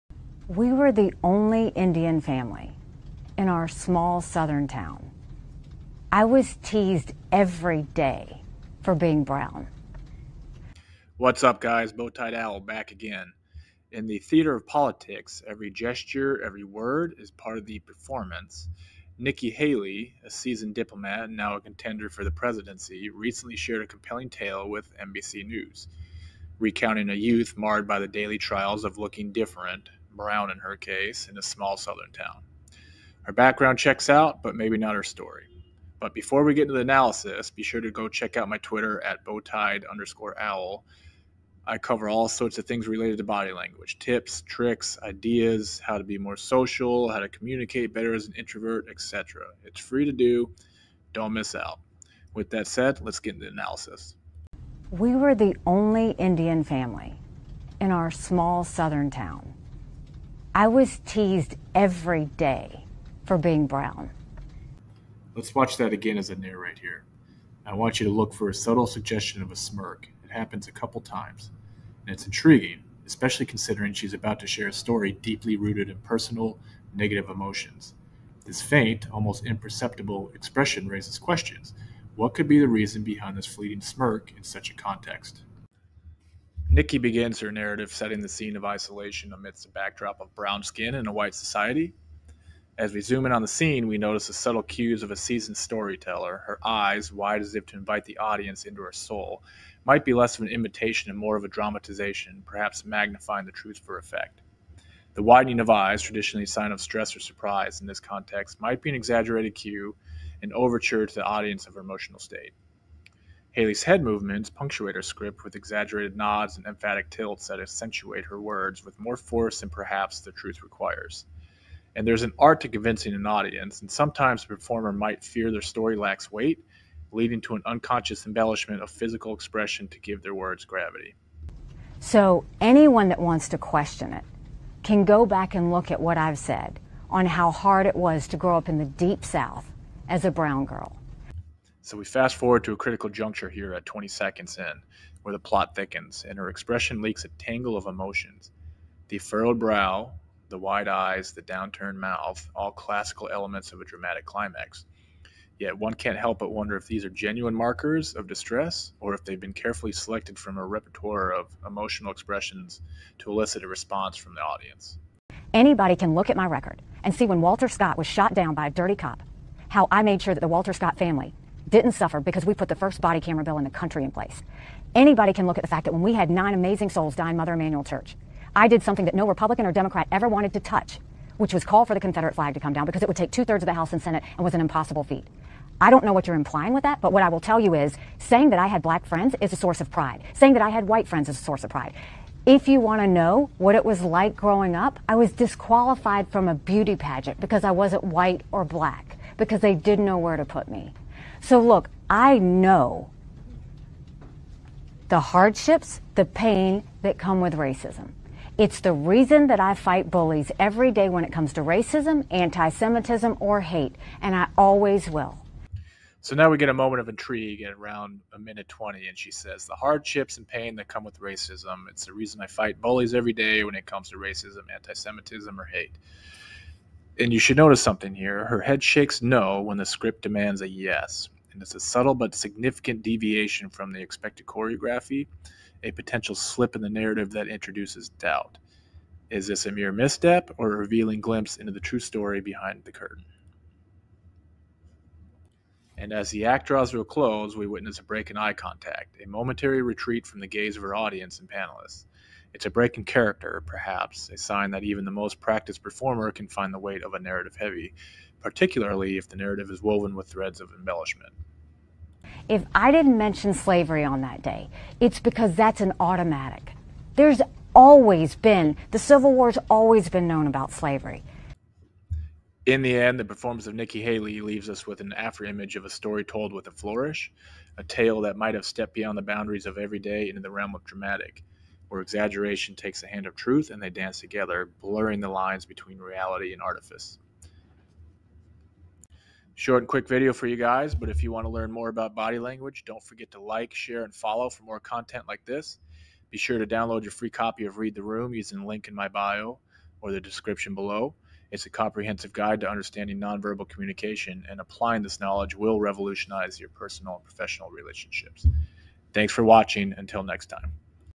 Fact or Staged? Nikki Haley Teased for Being Brown
Prepare for a perceptive deep-dive into the subtle intricacies of body language in the political sphere, spotlighting Nikki Haley. This video analysis will guide you through a riveting exploration of Haley's non-verbal communication during a notable NBC News interview. We'll closely examine how her gestures, expressions, and movements may not just be spontaneous reactions but potentially strategic elements designed to resonate with her audience.

As we dissect her performance, you'll gain insights into the subtleties of her body language – from the widening of her eyes that could signify an appeal to the viewer's empathy, to the faint smirks suggesting an underlying narrative. Watch as we parse the meaning behind her emphatic nods and tilts, and question whether these are heartfelt displays of emotion or a calculated enhancement of her story.

The highlight of our analysis will be the pivotal moment where her verbal affirmations seem to contradict her head shakes – a captivating instant that may reveal more than intended. We invite you to join us for this thorough breakdown, which promises to reveal the complex dance between crafted persona and genuine sentiment.